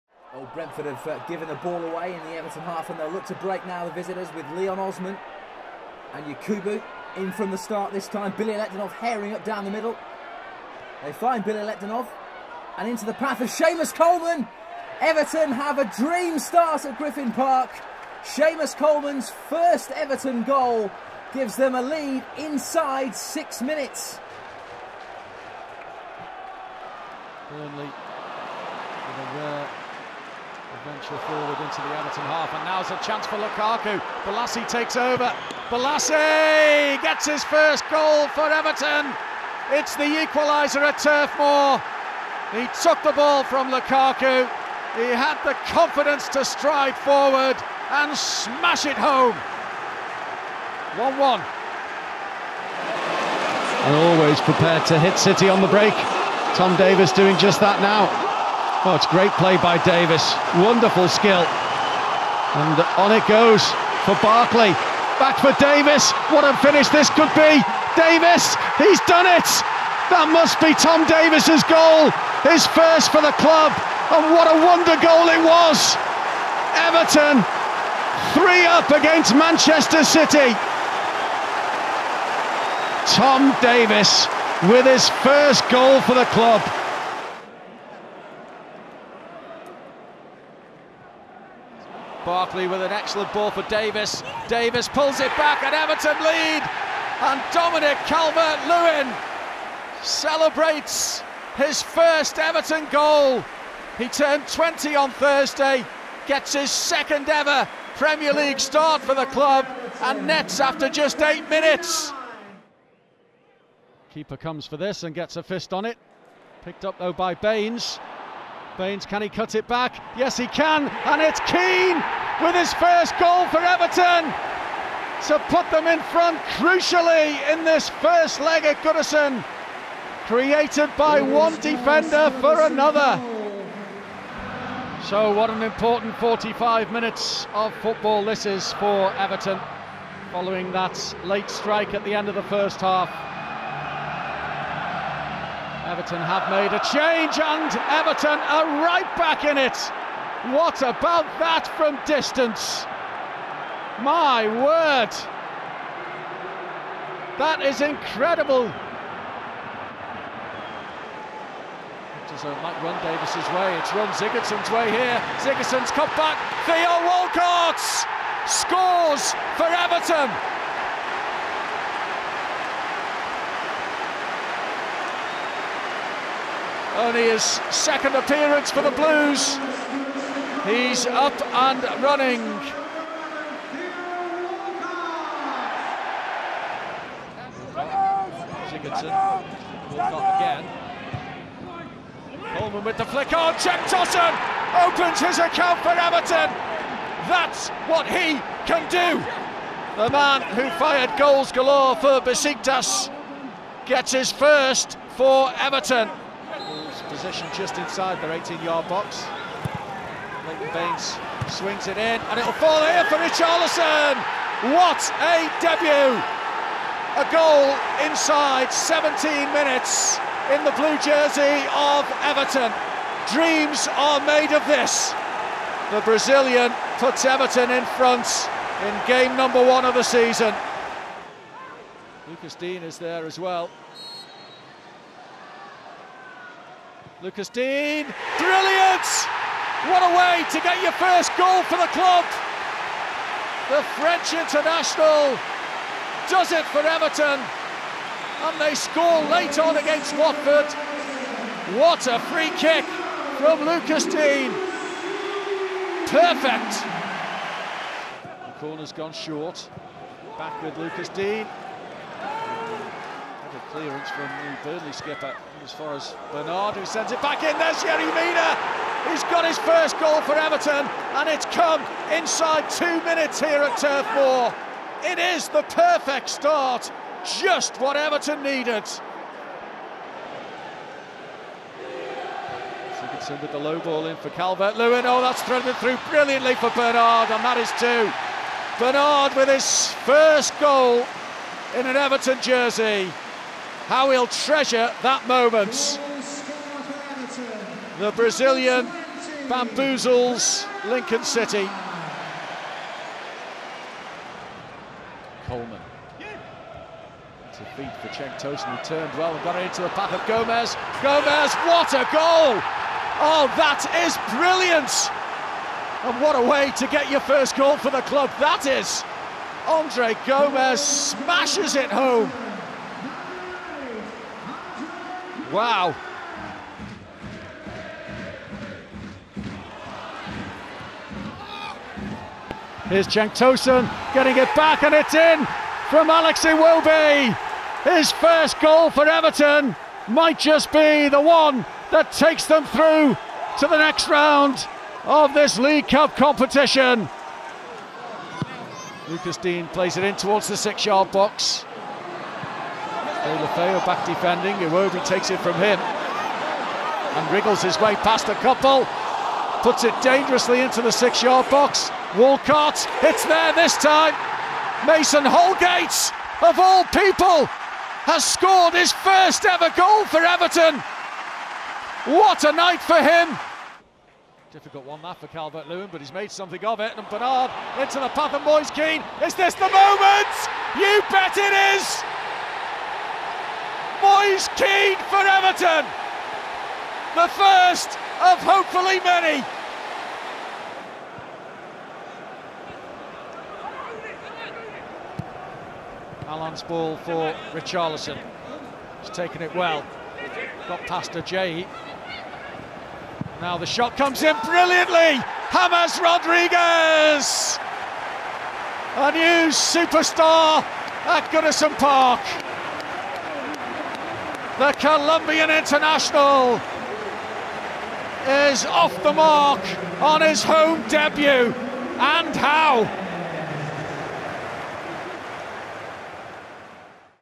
FIRST EVERTON GOALS! | FROM SEAMUS COLEMAN TO JAMES RODRIGUEZ
James Rodriguez netted his first Everton goal in the Blues’ latest game - and so we decided to take a chronological look at when the rest of the current squad opened their goal scoring account, starting all the way back in 2010 with Seamus Coleman!